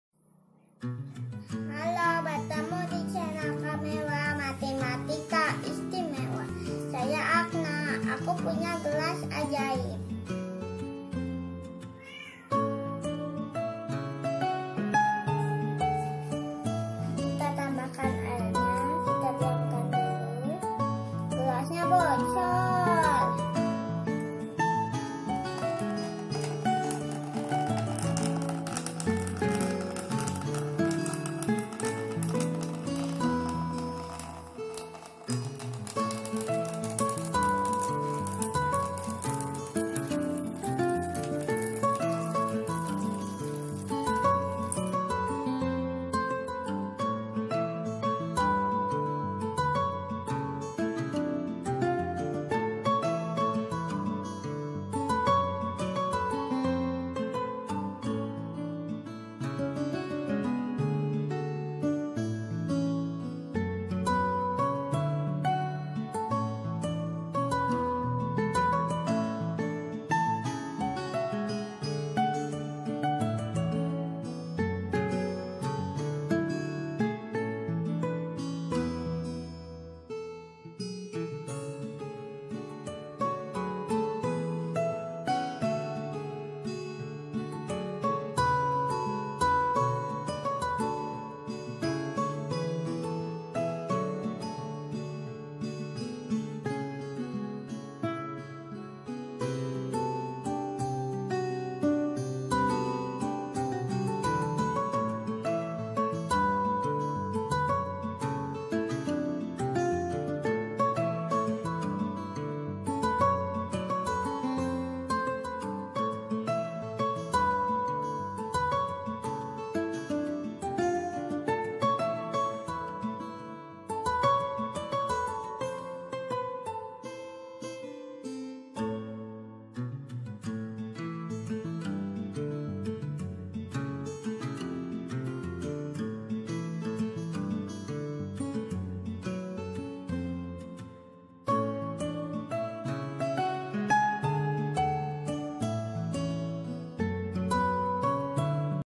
Gelas Ajaib Phytagoras
Atraksi gelas ajaib dan memahami debit air